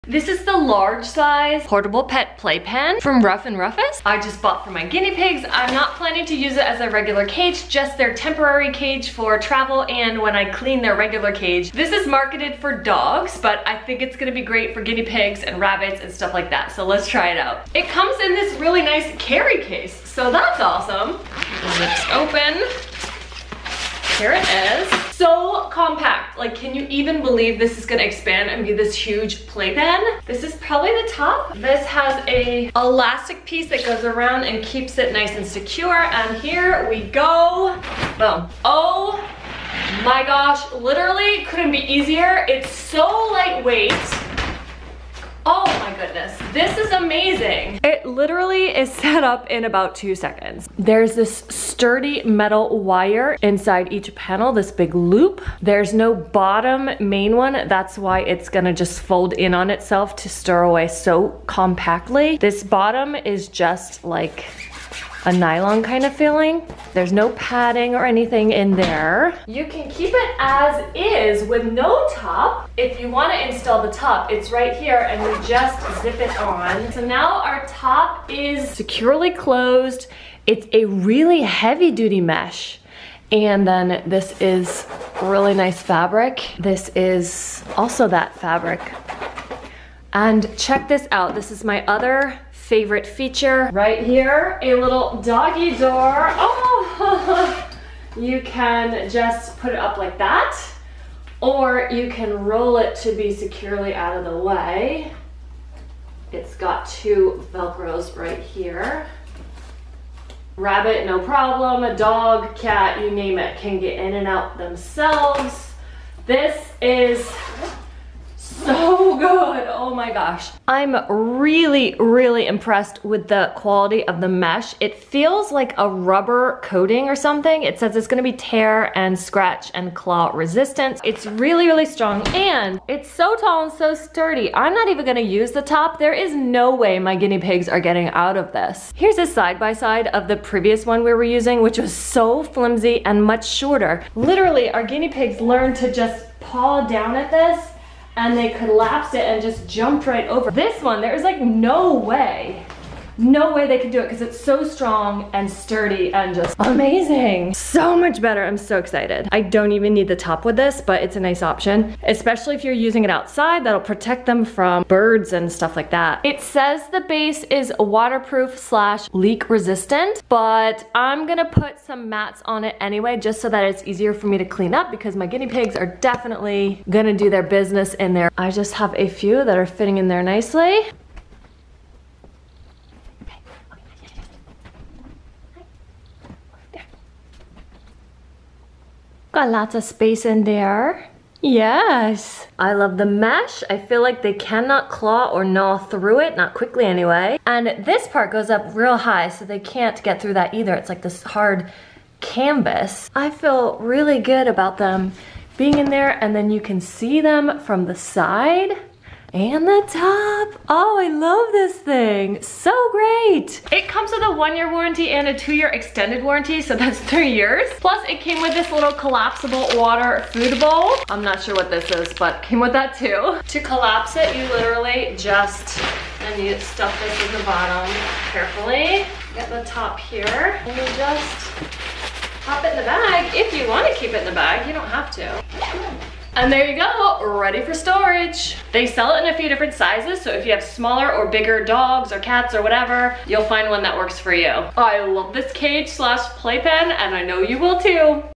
Ruff + Ruffus Pet Playpen - Full Demo + Review (for guinea pigs!)
Buy the Ruff 'N Ruffus Portable Foldable Pet Playpen for dogs, guinea pigs and other pets here (affiliate link) - I bought this myself for my guinea pigs temporary cage (when traveling or during our weekly cage deep cleaning) but as a Logie and Amazon Associate, I receive commission from purchases made after clicking the above affiliate link.

It features a doggy door and a removable roof/cover, is super lightweight, and literally takes just 30 seconds to assemble or disassemble. The side mesh is incredibly tough and the fabric is a heavy duty canvas, so it's going to be very difficult (impossible??) for small animals to scratch / chew their way out of it.